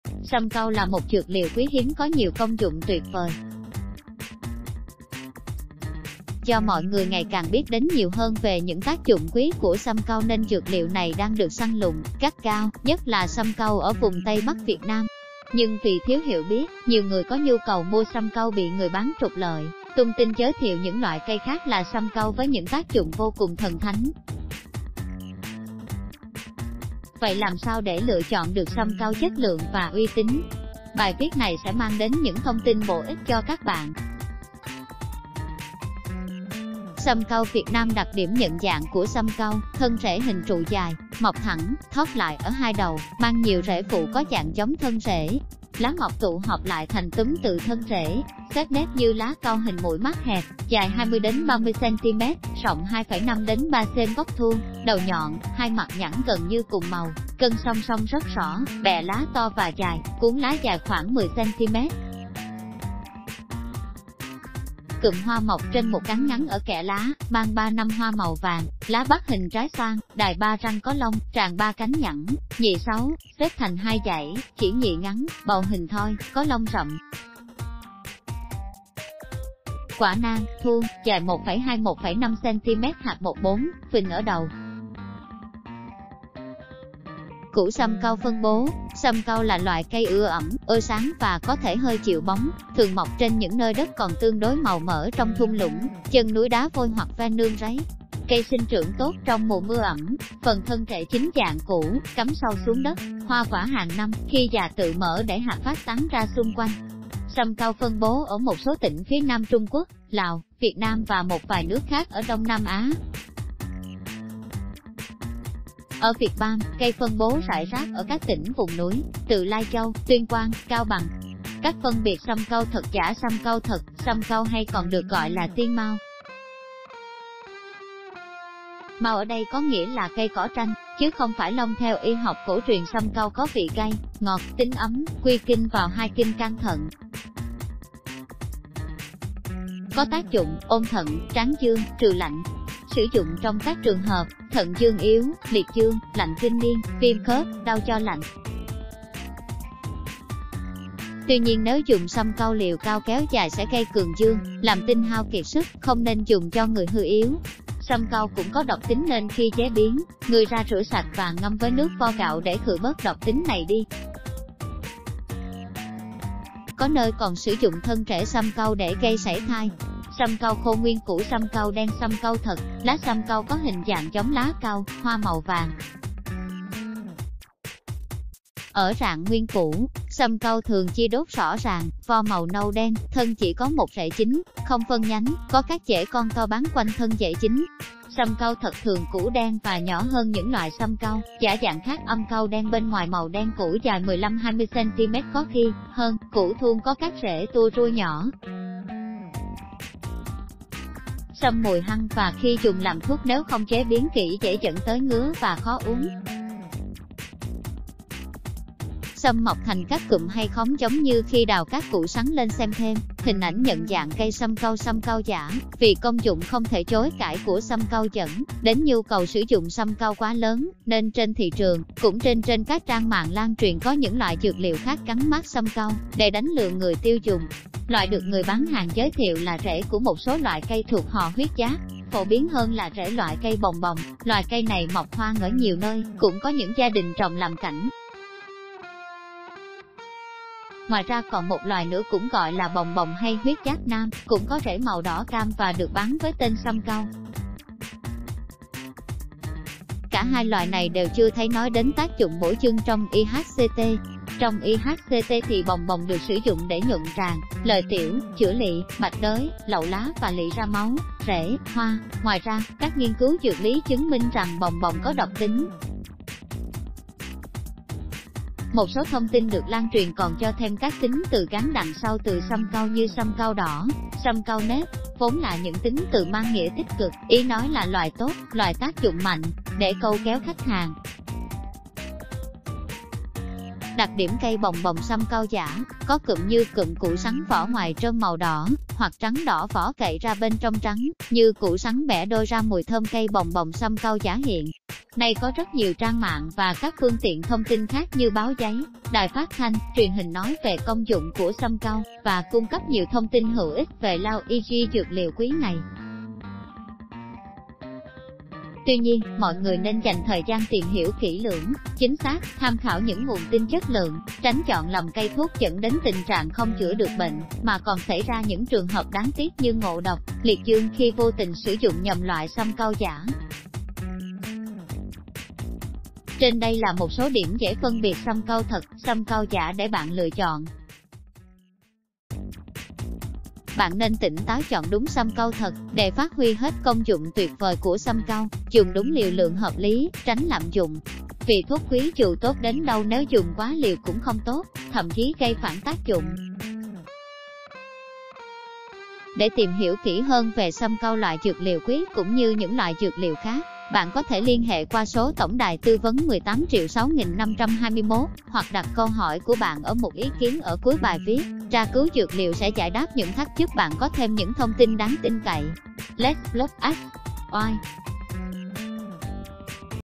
Phân biệt sâm cau thật, giả
Sâm cau là một dược liệu quý hiếm có nhiều công dụng tuyệt vời. Do mọi người  ngày càng biết đến nhiều hơn về những tác dụng quý của sâm cau nên dược liệu này đang được “săn lùng” gắt gao, nhất là sâm cau ở vùng Tây Bắc Việt Nam. Nhưng vì thiếu hiểu biết, nhiều người có nhu cầu mua sâm cau bị  người bán trục lợi, tung tin giới thiệu những loại cây khác là sâm cau với những tác dụng vô cùng thần thánh. Vậy làm sao để lựa chọn được sâm cau chất lượng và uy tín? Bài viết này sẽ mang đến những thông tin bổ ích cho các bạn. Sâm cau Việt Nam Đặc điểm nhận dạng của sâm cau: Thân rễ hình trụ dài, mọc thẳng, thót lại ở hai đầu, mang nhiều rễ phụ có dạng giống thân rễ. Lá mọc tụ họp lại thành túm từ thân rễ, xếp nếp như lá cau hình mũi mác hẹp, dài 20-30cm, rộng 2,5-3cmgốc thuôn, đầu nhọn, hai mặt nhẵn gần như cùng màu, gân song song rất rõ; bẹ lá to và dài; cuống lá dài khoảng 10 cm. Cụm hoa mọc trên một cán ngắn ở kẽ lá, mang 3 – 5 hoa màu vàng, lá bắc hình trái xoan, đài 3 răng có lông; tràng...